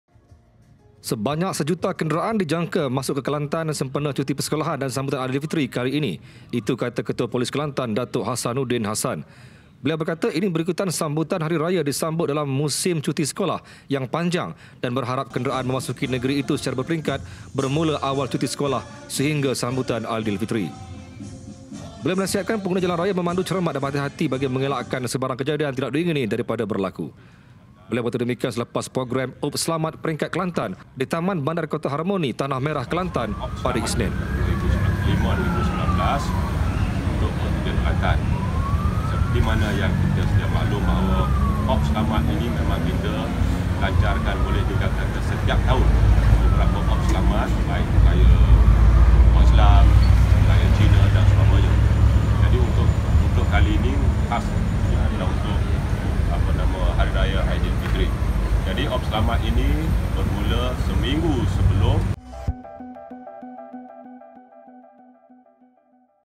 1 juta kenderaan masuk Kelantan Aidilfitri ini
Sejuta kenderaan dijangka masuk Kelantan kerana sambutan hari raya dalam musim cuti sekolah yang panjang